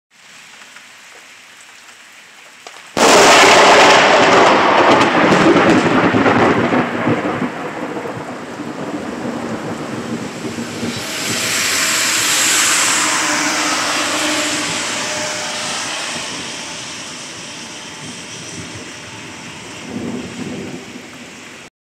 Close lighting hit. 9/28/21. Greene County PA.
I no sooner started filing and Lighting struck something about 50 yards behind my house.